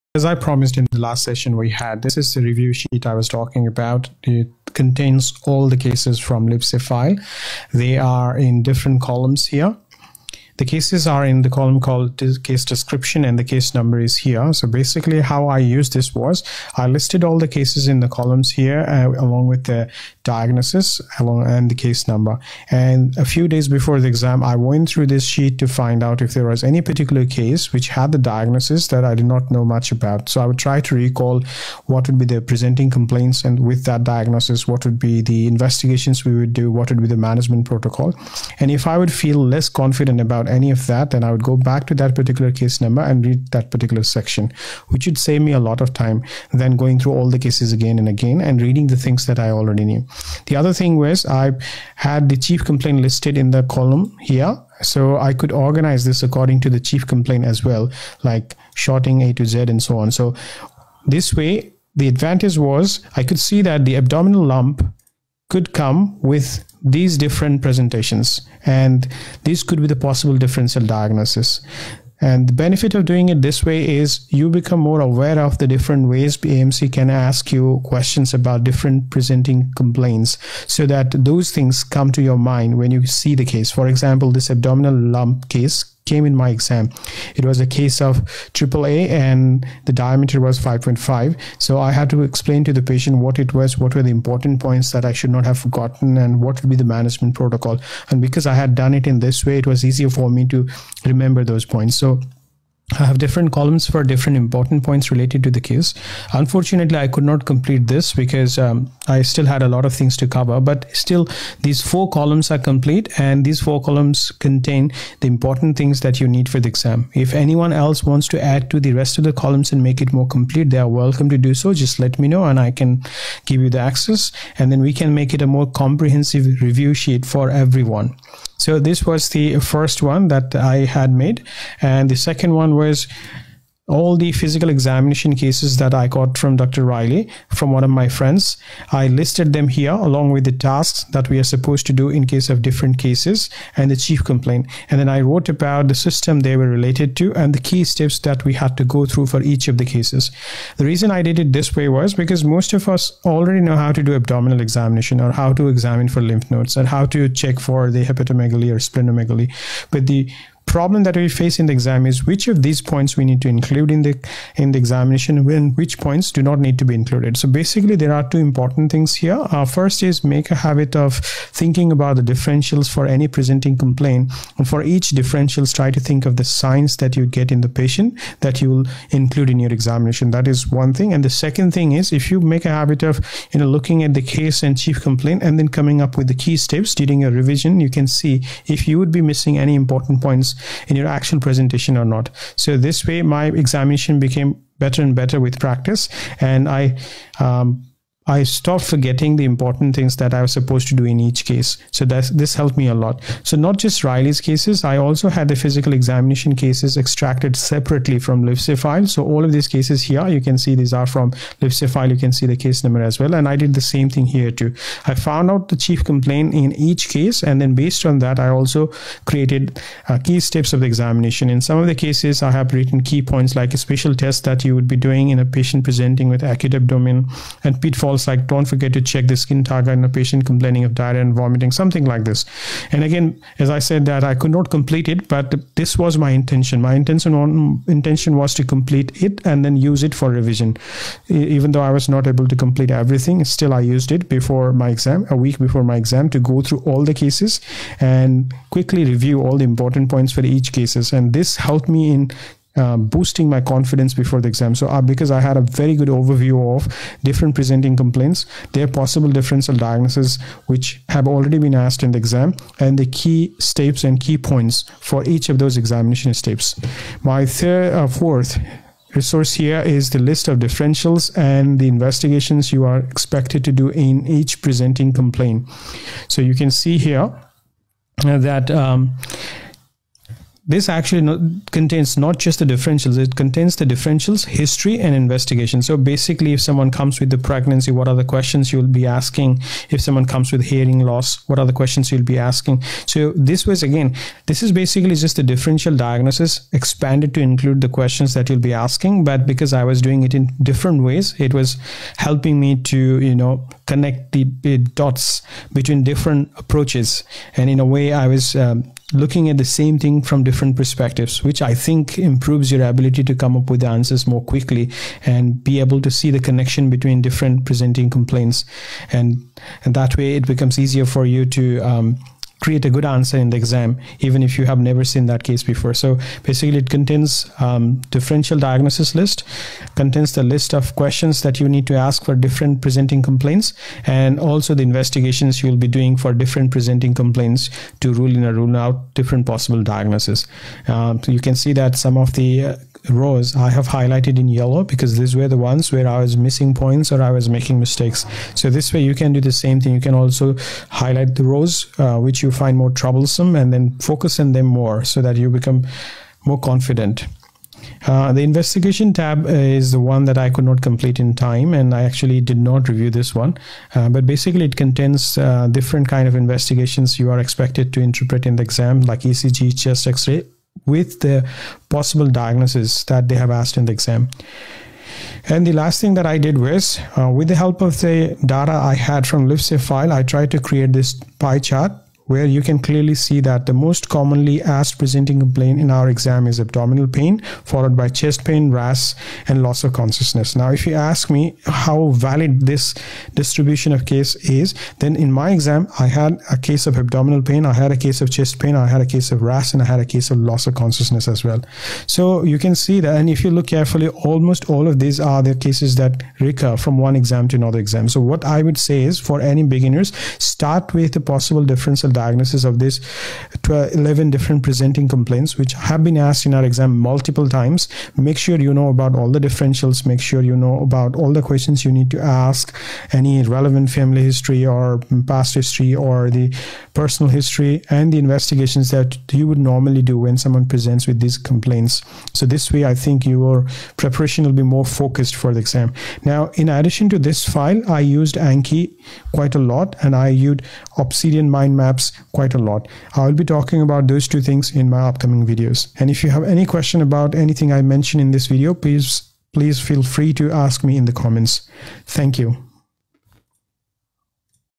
AMC Clinical Exam Review Sheet
I used a google sheet to list all the cases asked in AMC clinical exam to use it as a comprehensive review material. I am sharing this material freely. Use the link below to access it and use is as you wish.